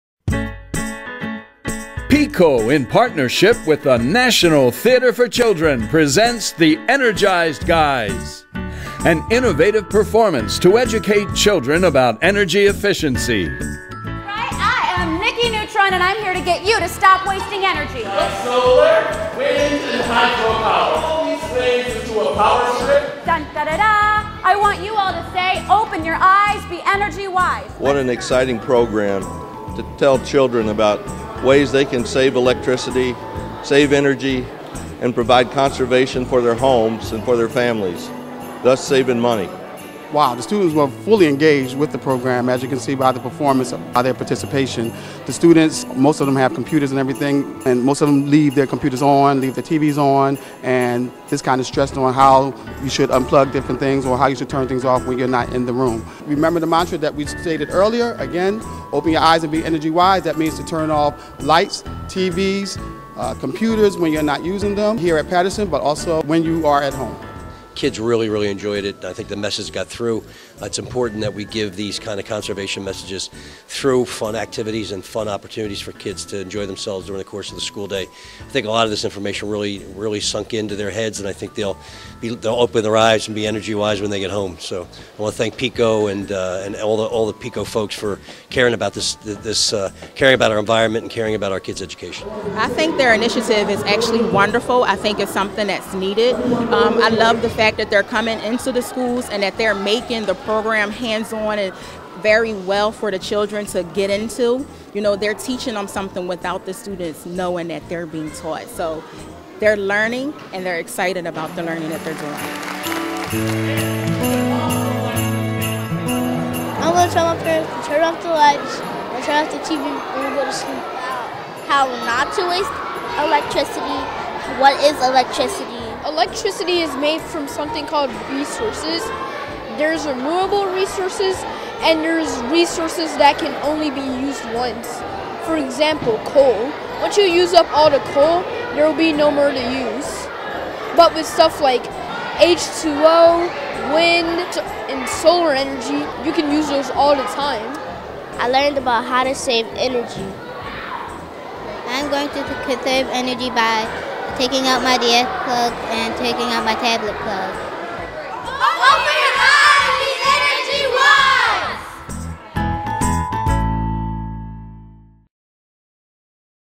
PECO & National Theatre for Children — The Energized Guyz
PECO provides lessons on energy efficiency to local elementary school students with productions of the National Theatre for Children's "Energized Guyz."

An audio transcript is available for this video. Please click here to open it